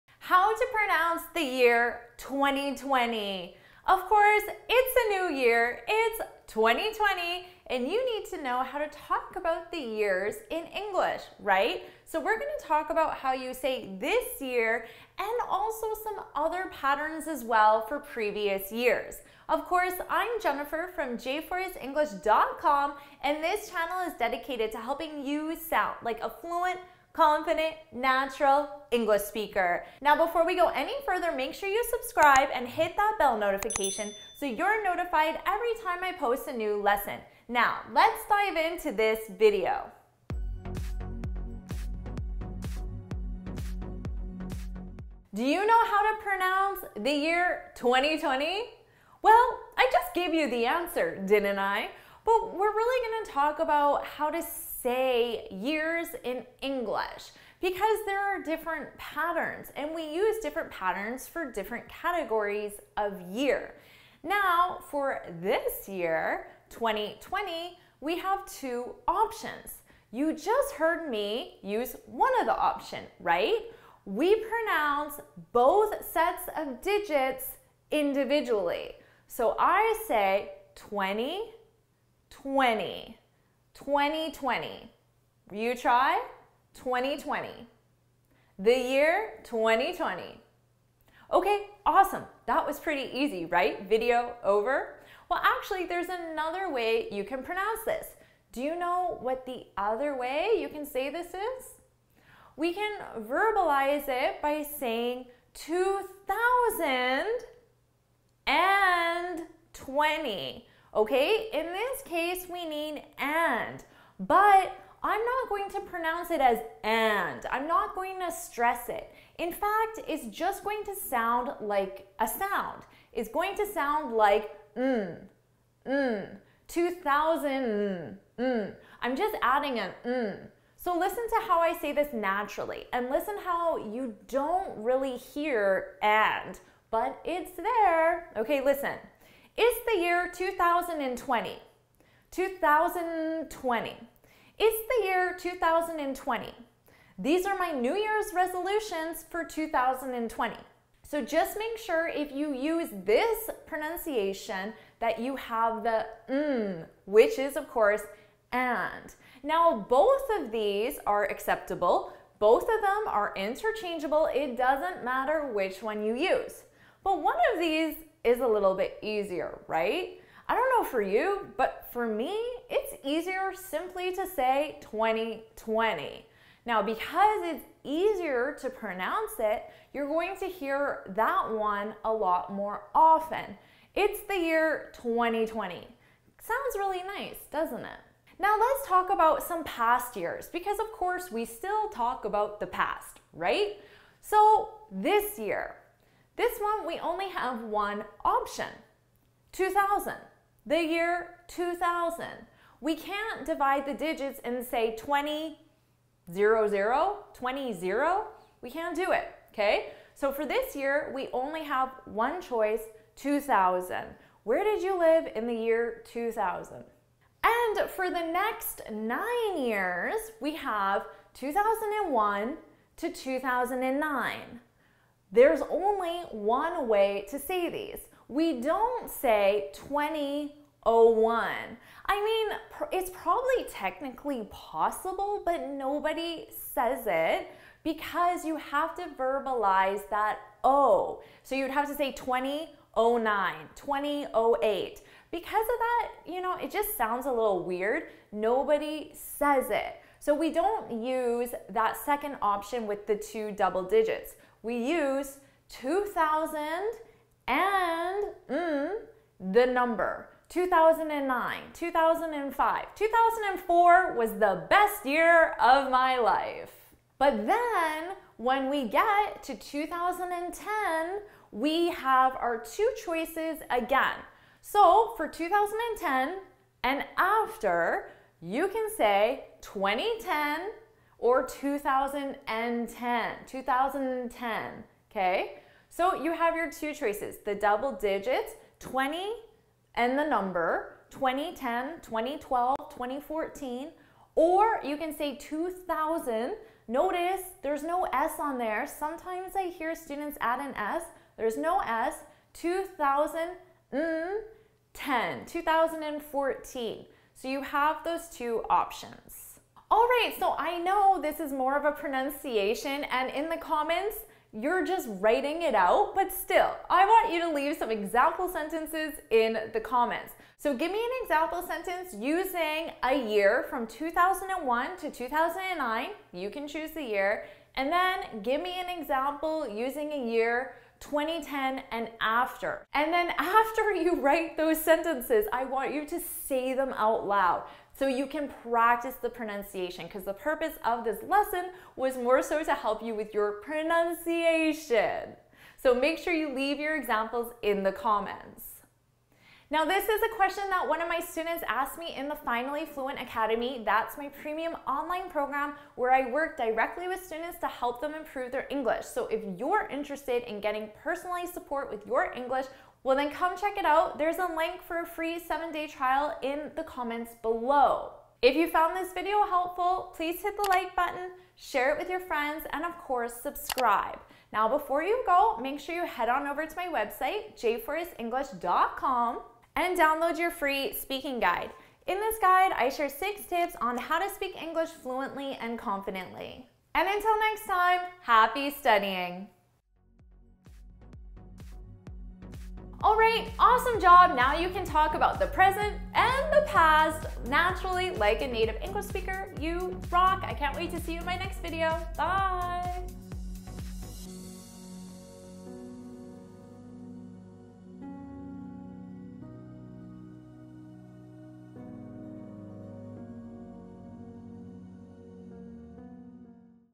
How to Pronounce 2020 in English - How to Say Years in English
In this video, learn how to pronounce 2020 in English.

You'll learn two different ways of saying these numbers out loud.

The first is to divide them into two sets of digits. You'd say: twenty twenty.

The second is to say: two thousand and twenty. Here you have to use "and", but it's pronounced very softly. I'll teach you how to pronounce it like a native English speaker.

The first option is a lot easier to say, even for native speakers. Because of that, it's the more common option.

But, it's not possible to use the first option for all years in English.

From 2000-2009, we only use the second option (two thousand and #).

From 2010 and beyond, we have two options.

By the end of the video, you'll feel confident taking about years in English.